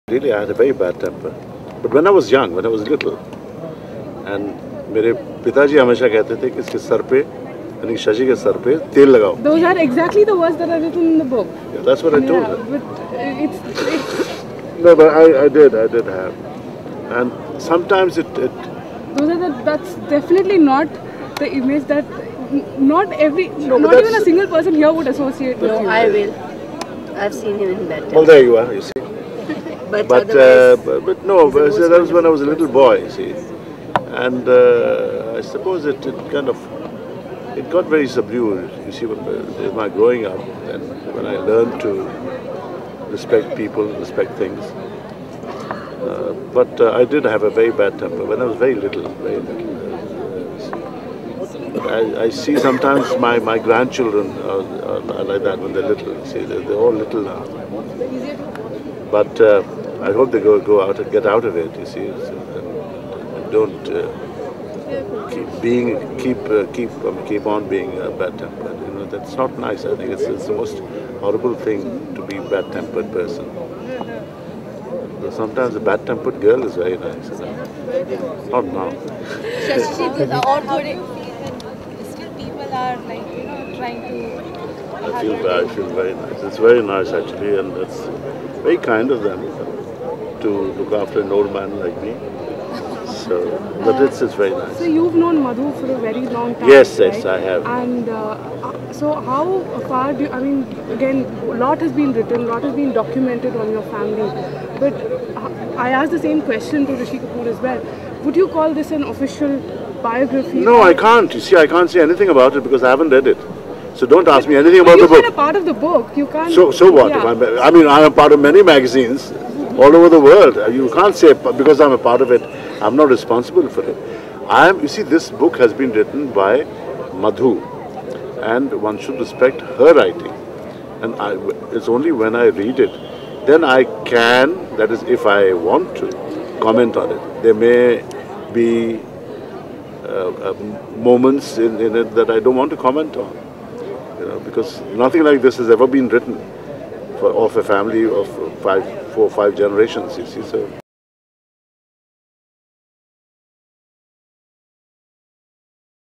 Sashi Kapoor: "I had a really bad temper. Sometimes a bad tempered girl is very nice!"
Sashi Kapoor, while blowing his nose with a handkerchief: "I had a really bad temper. When I was little, my father used to say that oil needs to be put on my head.  I suppose it got subdued when I grew up and learned to respect people and things. I see sometimes my grandchildren. I hope they get out of it, and keep on being bad tempered. It's the most horrible thing to be bad tempered. Sometimes a bad tempered girl is very nice! I have known Madhu for a long time. This book has been written by Madhu and one must respect her writing. There may be moments that I don't want to comment on."

Sashi Kapoor: "I had a really bad temper"